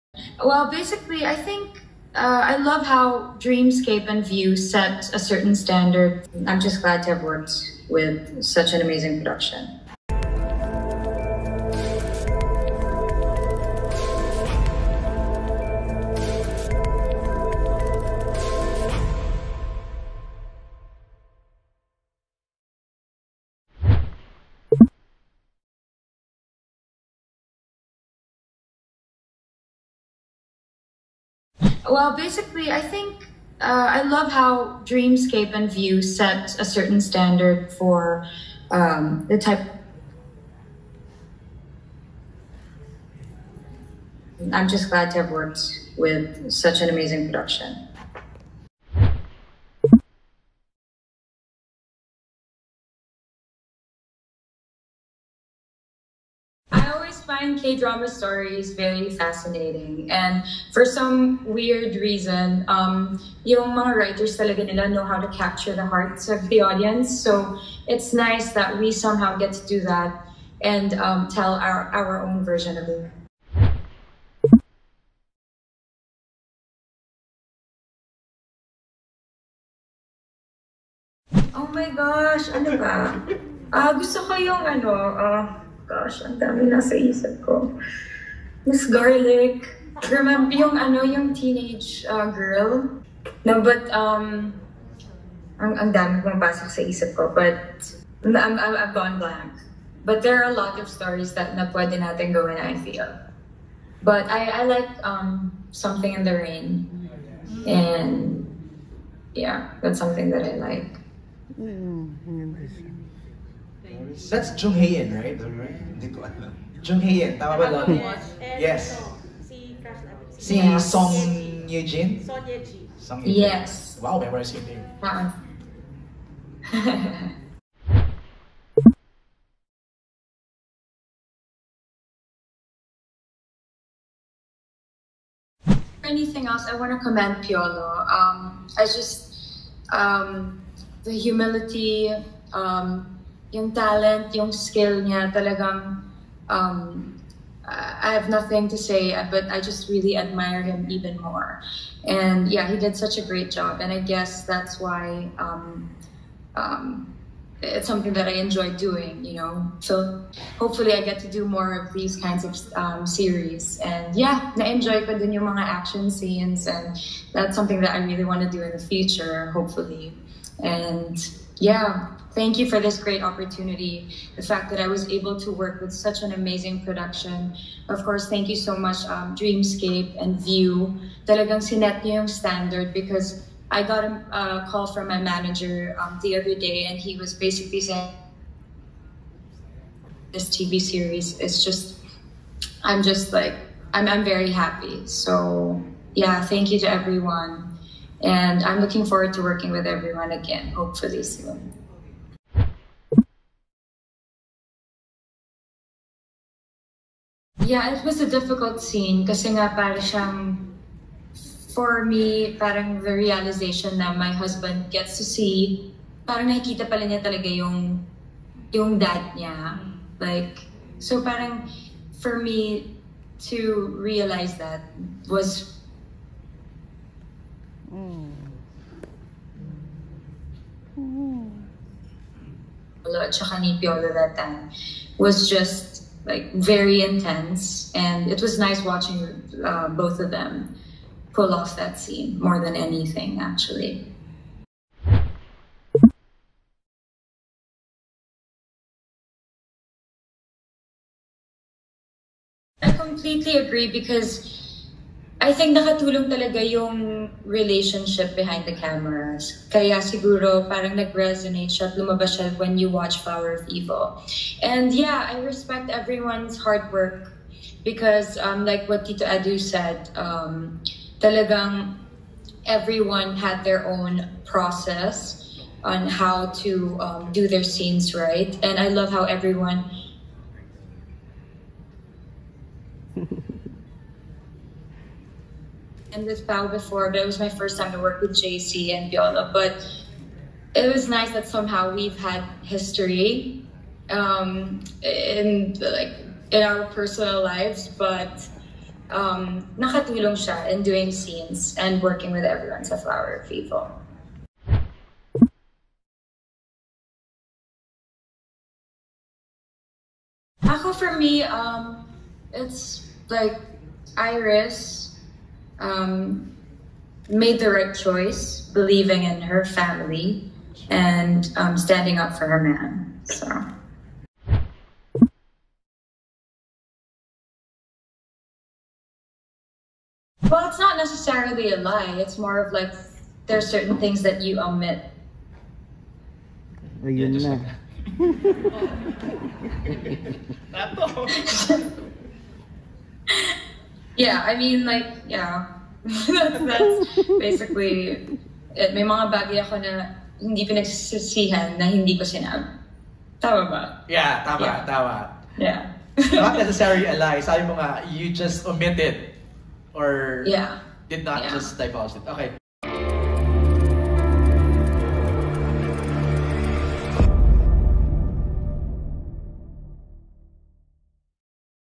Lovi Poe on how Dreamscape and Viu set a high standard for 'Flower of Evil'
At the media conference, including LionhearTV, on October 4, Kapamilya actress Lovi Poe highlighted how Dreamscape and Viu set a high standard for the cast and production team of Flower of Evil, the Filipino adaptation of the Korean series of the same title.